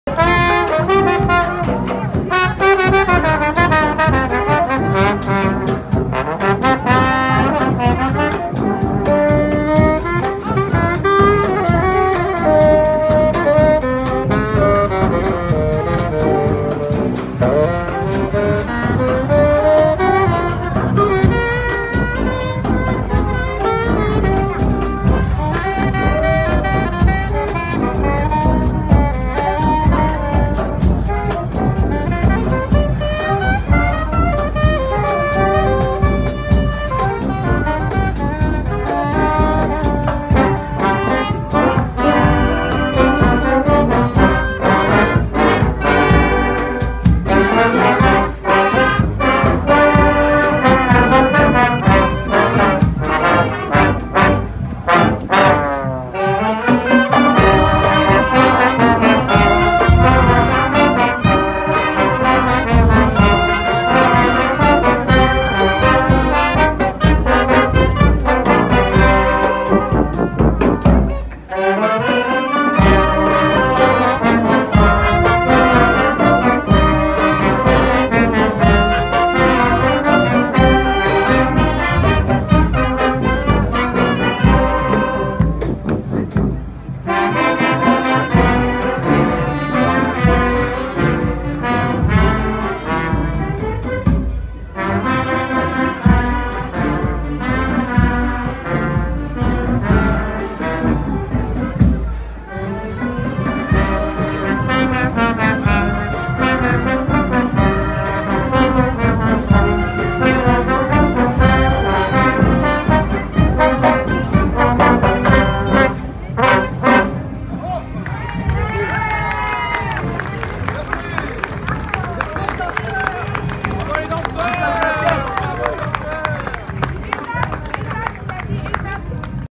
LaFoireAuxCroûtesBrest2022 place Guerin ce samedi 28 mai la DER
en ouverture, le Festival des Fanfares, par ce beau soleil brestois